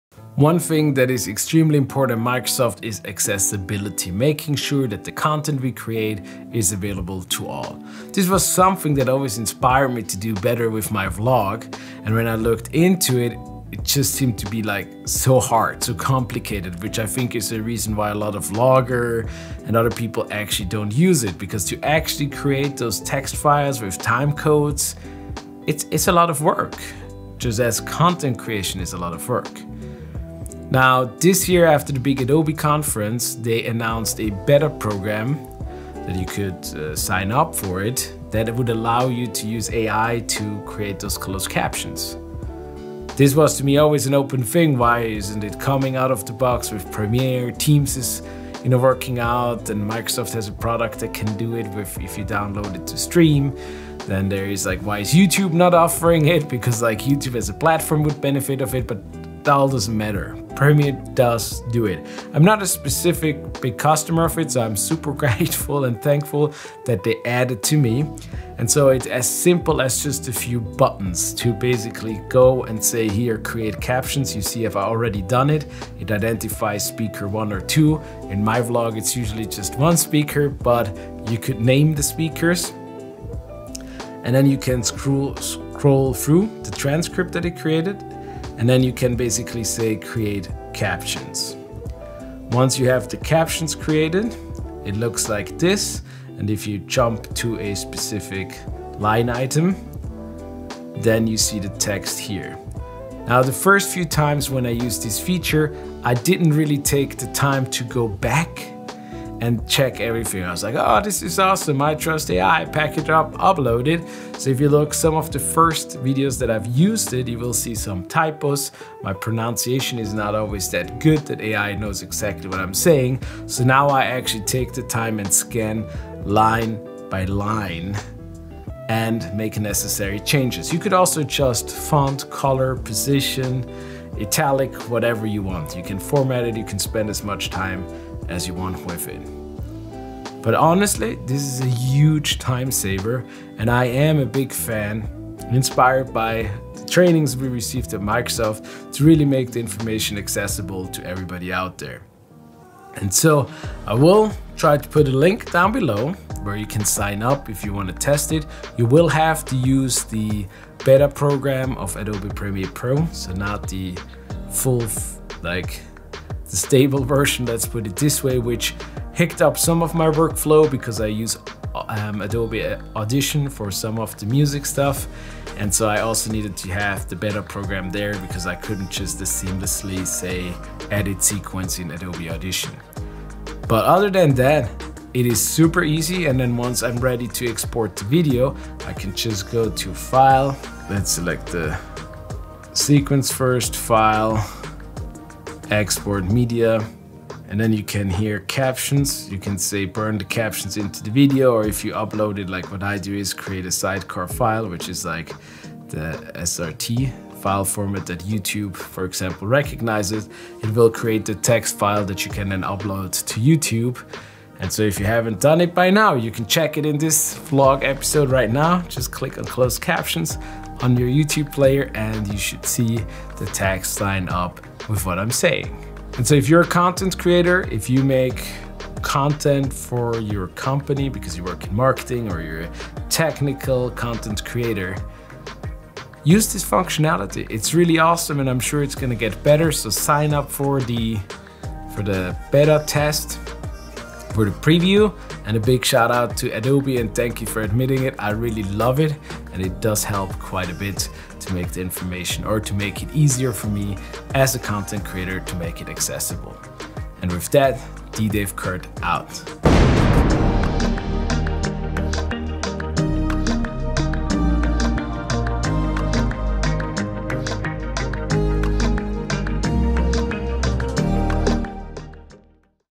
VLOG #81 - Adobe Premiere Pro leverages AI to create closed caption
it never has been as easy for content creators to create closed caption than with the new beta feature of Adobe Premiere Pro. You can sign up to their beta